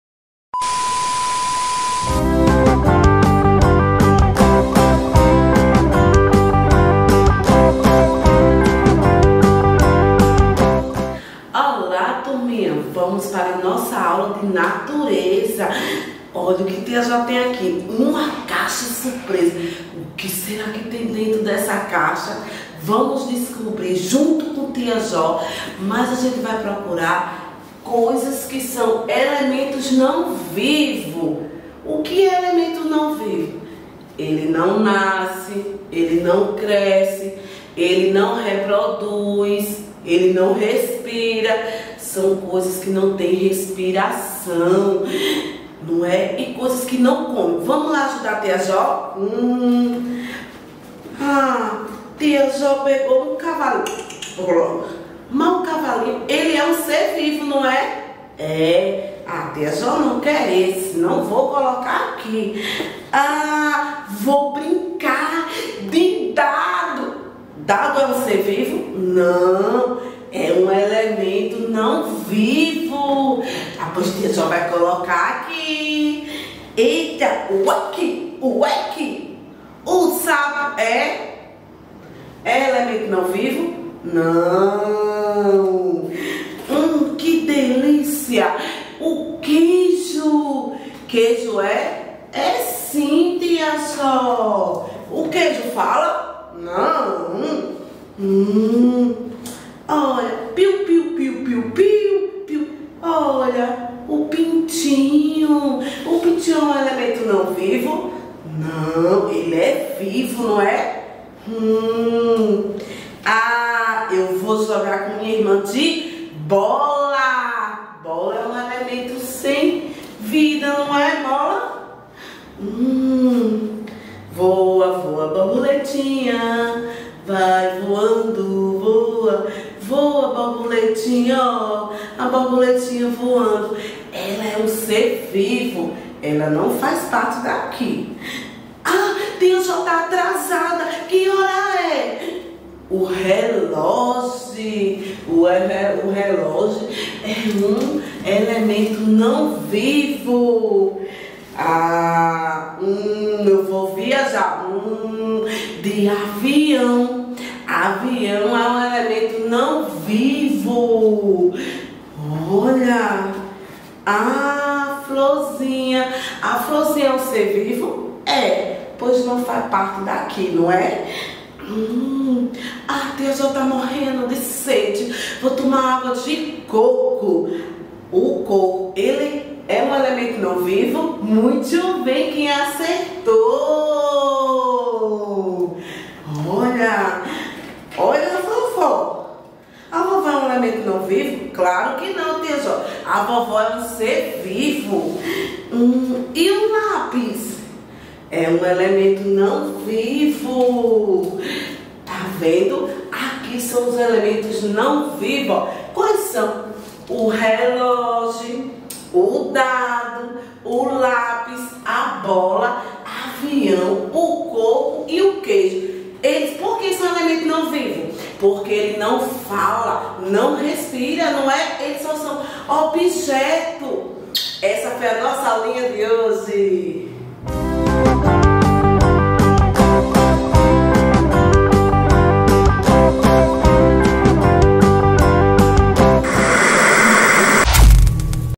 PRÉ ESCOLAR 4 ANOS NATUREZA ELEMENTOS NÃO VIVOS AULA PARA 15102021
PRÉ ESCOLAR 4 ANOS NATUREZA ELEMENTOS NÃO VIVOS AULA PARA 15102021 /// Aula sobre os Elementos Não Vivos que existem na Natureza, suas características e importância;